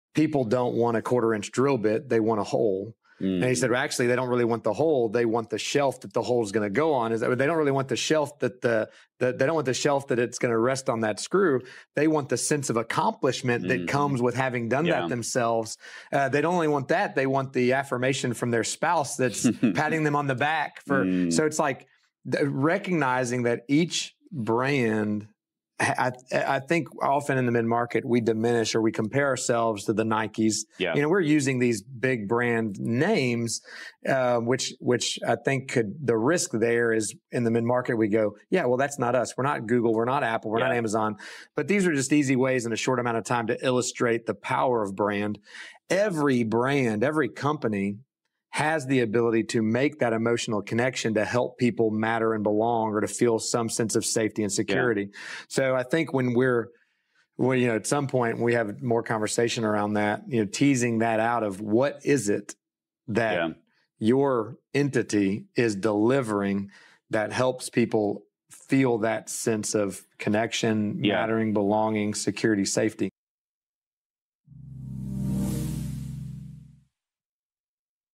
This Is The Power Of Your Brand
To make an emotional connection.

Everybody wants to matter and belong. Everybody wants to feel safe and secure, which is exactly what the best brands do. They create an emotional connection with their audience through telling a consistent story.

If this resonates, listen to our most recent podcast with brand strategy expert Jamin Jantz